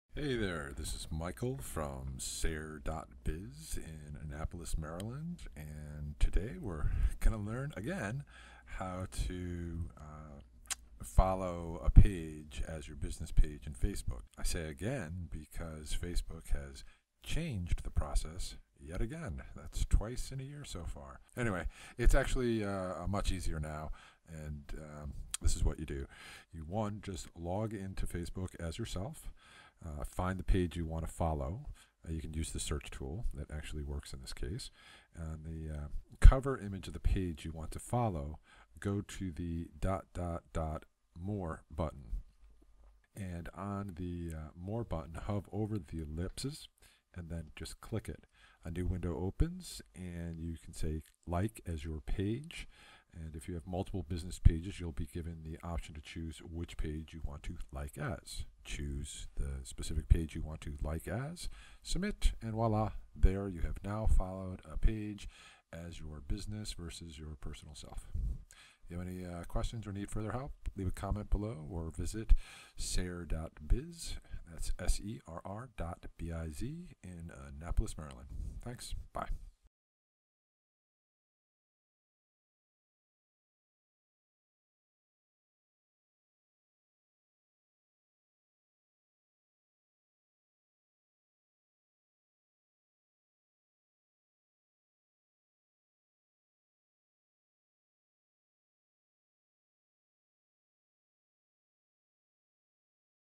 Facebook page follow another page Sept 2016 | Tutorial
Good old Facebook changed the process of how to follow a page as your business page. Again! That's twice in 6 months. This time, it's actually easier. Watch video to see how.